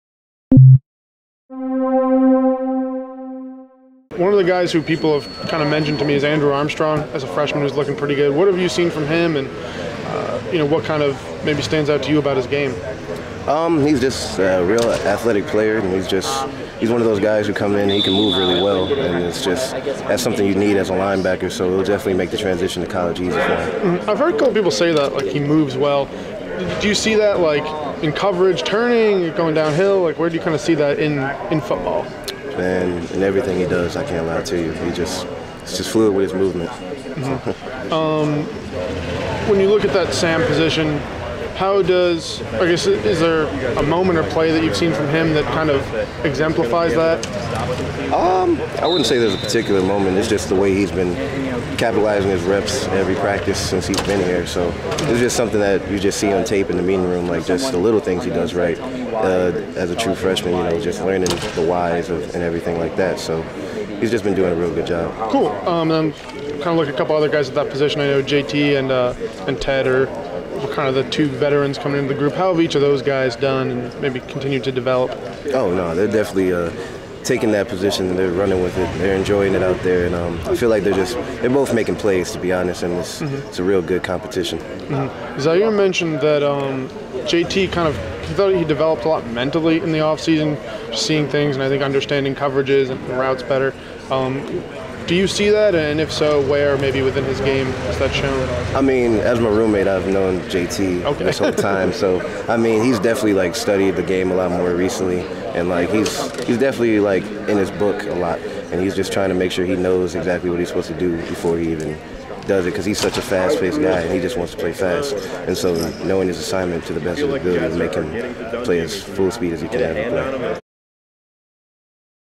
Syracuse football's Parris Bennett talks about freshman Andrew Armstrong
After today&apos;s Syracuse University football preseason practice at the Clifford J. Ensley Athletic Center, linebacker Parris Bennett talked about the play of freshman linebacker Andrew Armstrong.